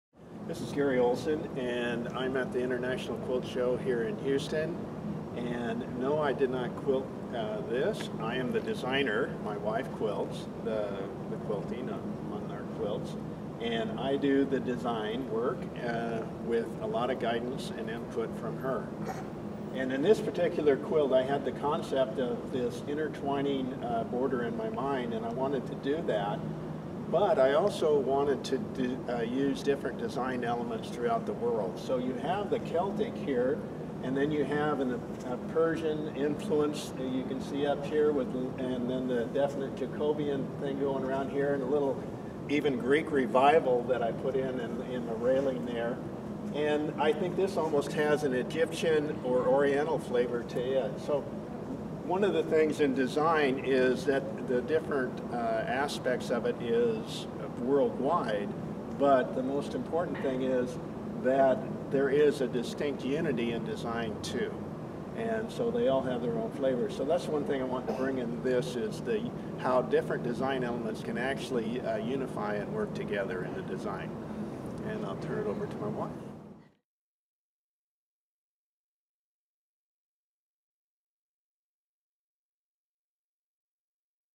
Go Tell It at the Quilt Show! interview with Garry Olson
"Go Tell It at the Quilt Show!" is a Quilt Alliance project designed to capture the stories of quilts where quiltmakers gather. Garry Olson was interviewed during the 2014 International Quilt Festival in Houston, Texas.